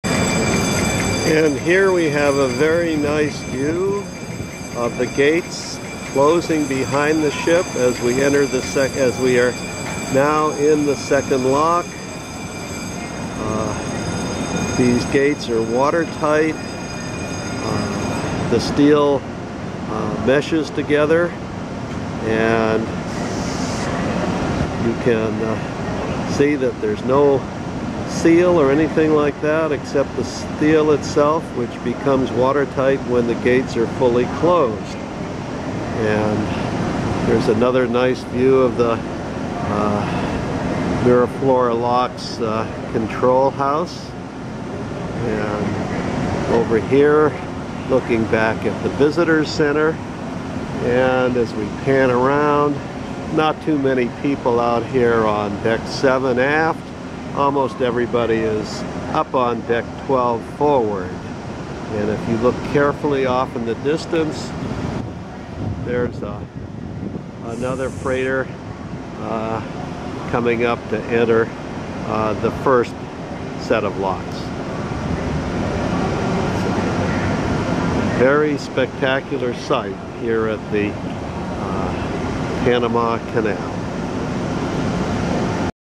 Panama Canal Transit - Sept. 2011 - Clip 4
Miraflores Lock gates closing behind Crystal Symphony.